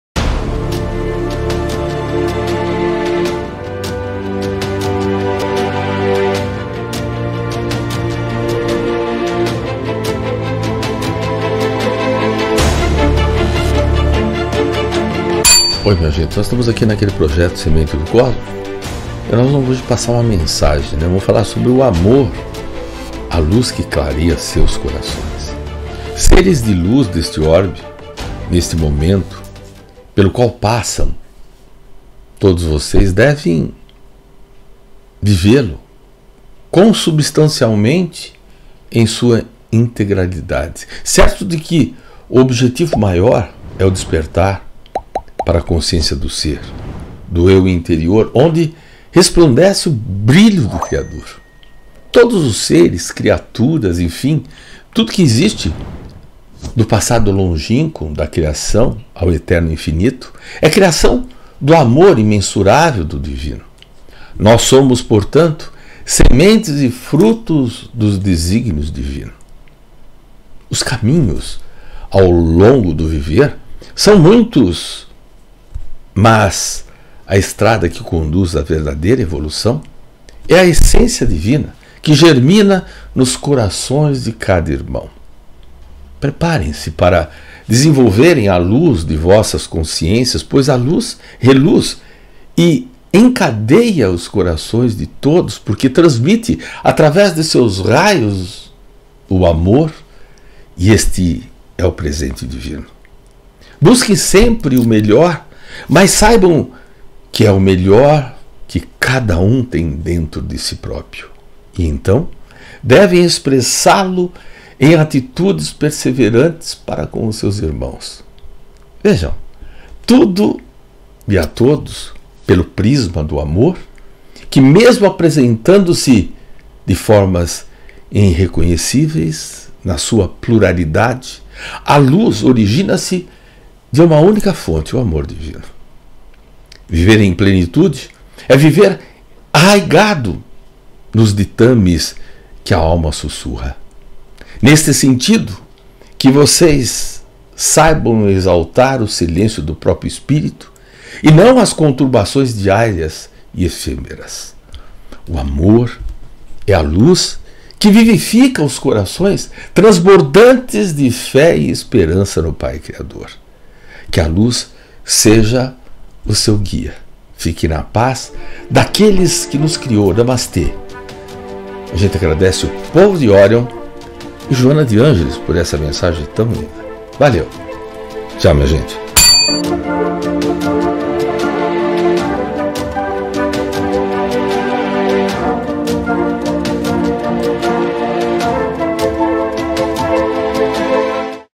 Amor, a Luz Que Clareia Seus Corações | Sinuhe da Silva Vieira-Sementes do Cosmo
Somos criação do amor imensurável do Divino
Veja esse vídeo! | Amor, a Luz Que Clareia Seus Corações | Sinuhe da Silva Vieira - Sementes do Cosmo, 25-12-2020

Lembre-se de "curtir" e ""compartilhar" com os seus amigos.
Agradecemos.